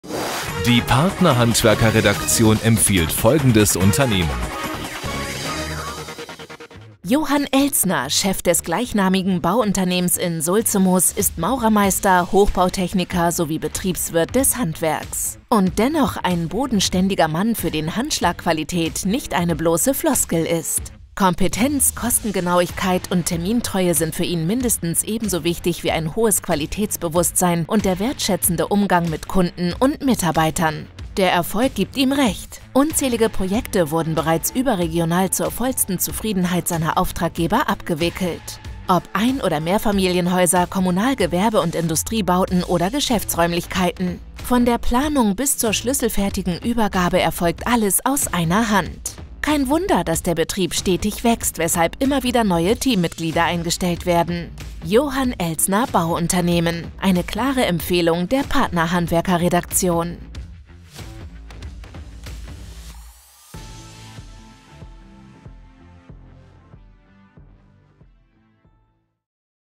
Vom Einfamilienhaus bis zum Gewerbeprojekt mit Johann Elsner Bauunternehmen in Sulzemoos
Der Betrieb von Johann Elsner in Sulzemoos gilt als Spezialist für Gesamtlösungen im Baubereich. Geboten werden sämtliche Leistungen von der Beratung und 2-D-/3-D-Planung bis hin zur schlüsselfertigen Übergabe für:
- Einfamilien-, Mehrfamilien- und Reihendoppelhäuser
- Neubauten
- Wohnbauprojekte
- Kommunalbauprojekte
- Gewerbeprojekte
- Geschäfts- und Büroräumlichkeiten
- Industriebauprojekte

0:00 PARTNERHANDWERKER.DE präsentiert
0:11 Maurermeister, Hochbautechniker & Betriebswirt
0:38 Ein- & Mehrfamilienhäuser
0:40 Kommunal-, Gewerbe- & Industriebauten

Für weitere Informationen zu Ihrem Bauprofi in Sulzemoos besuchen Sie auch die